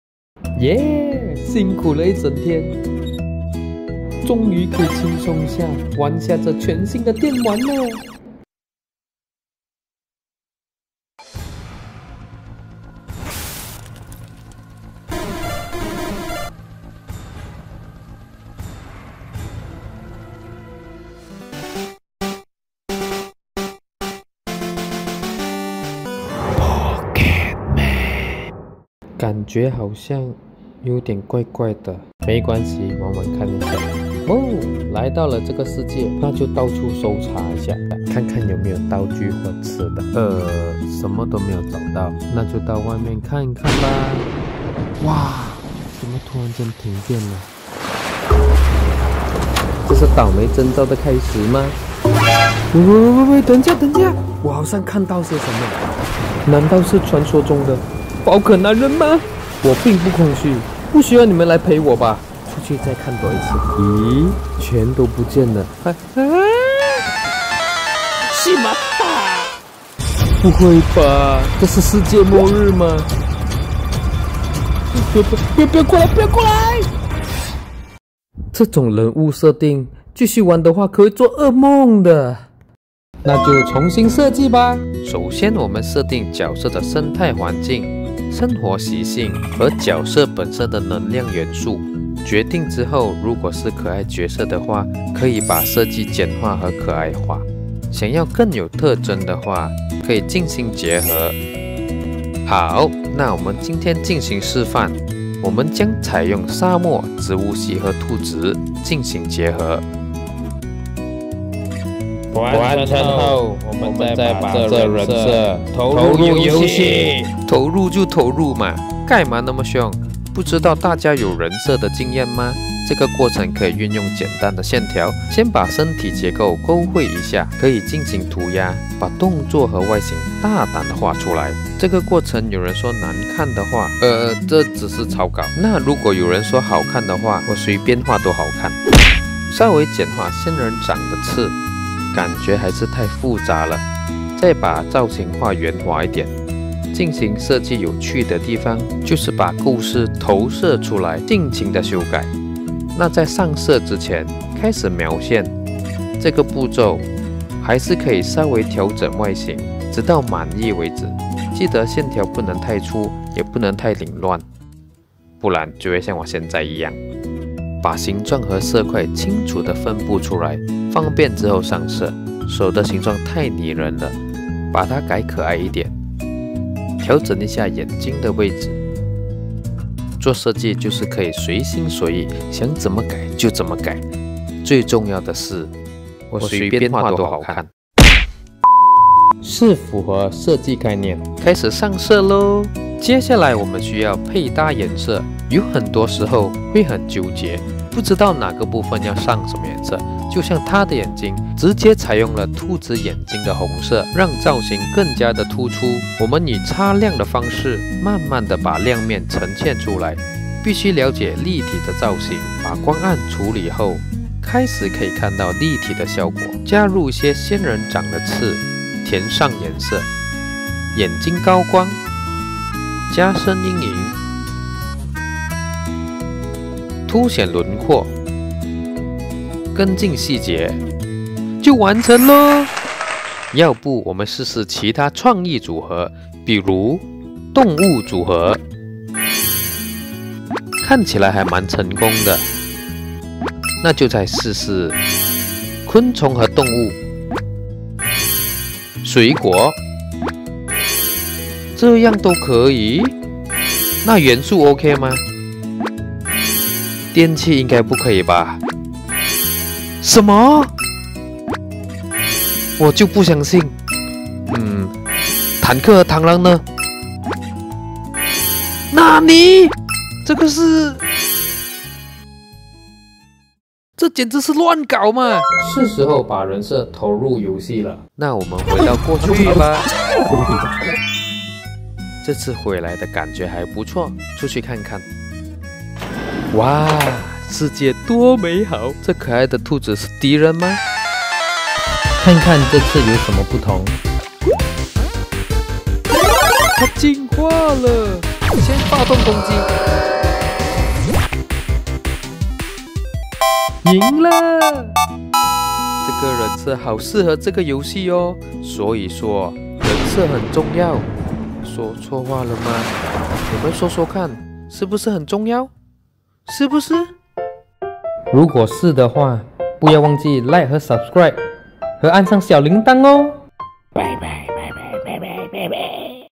五分钟教你画宝可梦！
恐怖电玩改成可爱宝可梦模拟过程